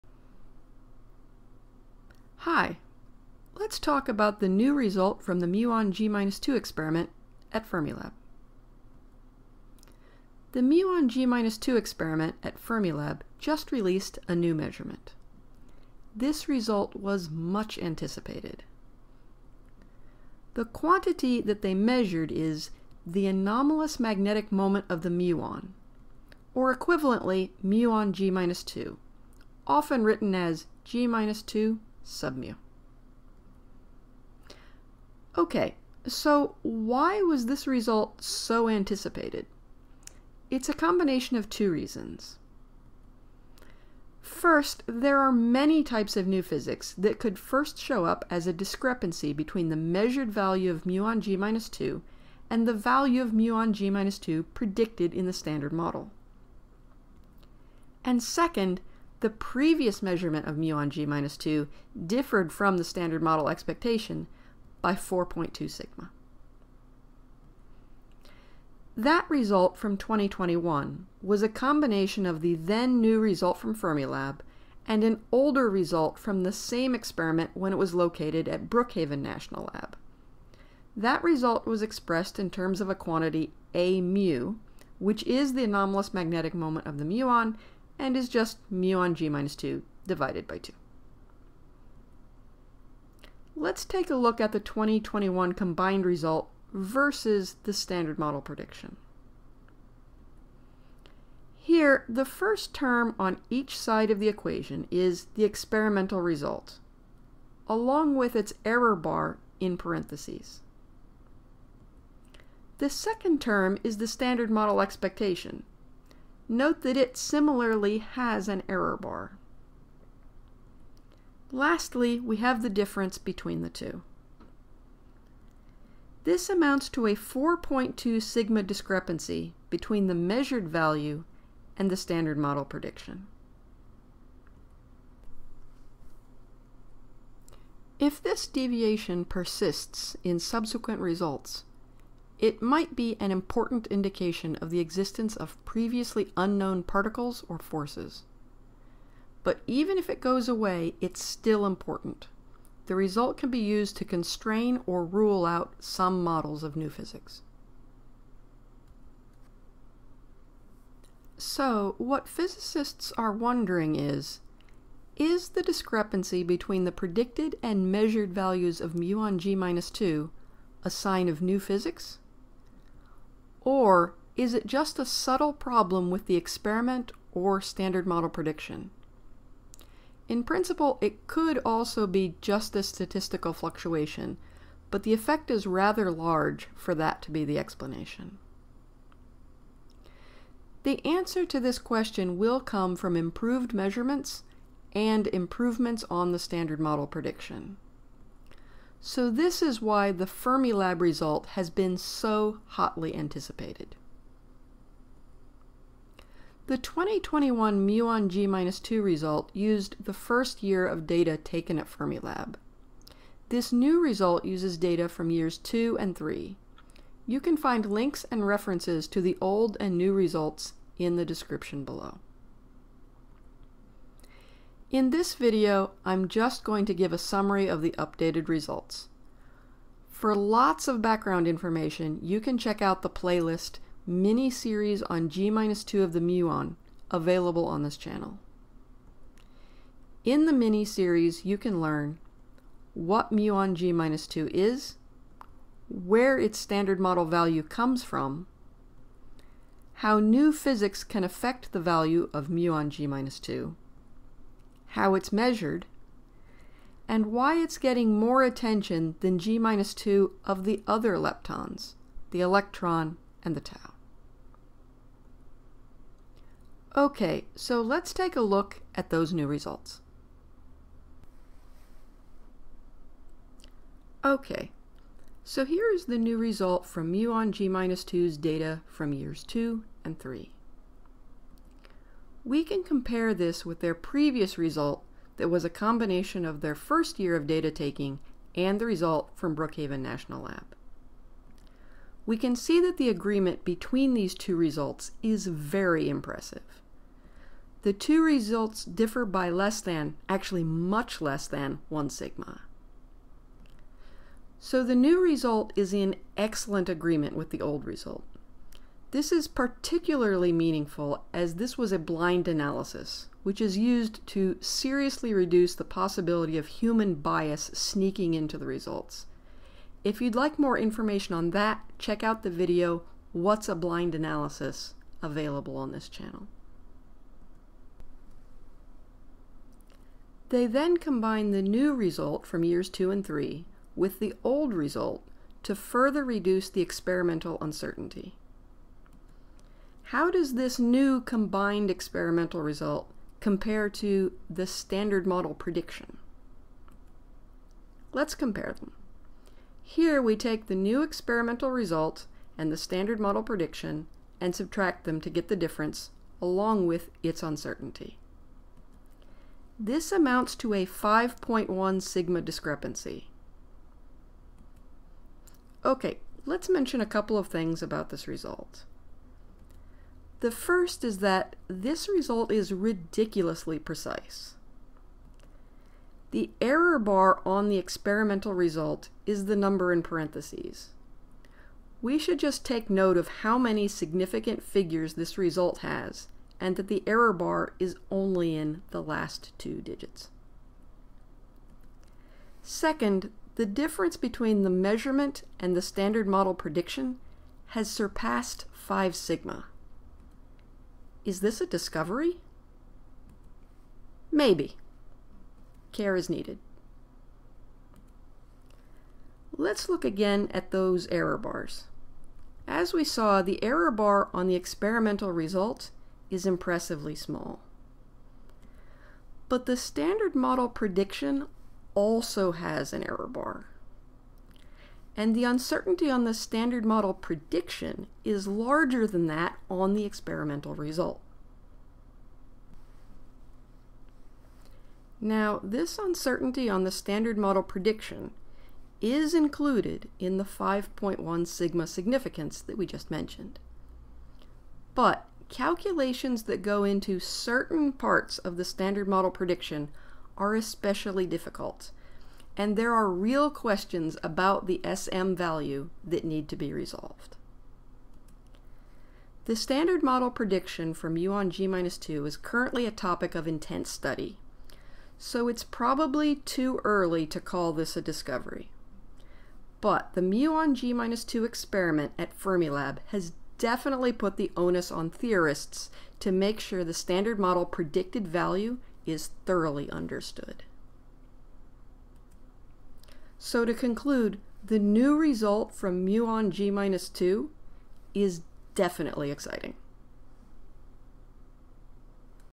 The New (2023) Result from the Muon g-2 Experiment
The Muon g-2 Experiment at Fermilab just released an updated measurement of the anomalous magnetic moment of the muon.  This is an update of their 2021 result which showed a deviation from the Standard Model prediction of 4.2 sigma.

There are many types of new physics that could first show up as a deviation in the measured value of muon g-2 from its value predicted in the Standard Model.  So, this result has implications for models of dark matter, supersymmetry, Higgs physics, and others.  For this reason, this updated result has been much anticipated.

For lots of background information on muon g-2, check out the Miniseries on g-2 of the Muon:

Fermilab's 2021 result:

2021 Papers:
Muon g-2 Collaboration, Measurement of the Positive Muon Anomalous Magnetic Moment to 0.46 ppm (2021)
B. Abi et al. Phys. Rev. Lett. 126, 141801

Muon g-2 Collaboration, Measurement of the anomalous precession frequency of the muon in the Fermilab Muon g-2 Experiment (2021)
T. Albahri et al. Phys. Rev. D 103, 072002

Aoyama, T., et al, The anomalous magnetic moment of the muon in the Standard Model (2020), Physics Reports 887, 3 December 2020, Pages 1-166.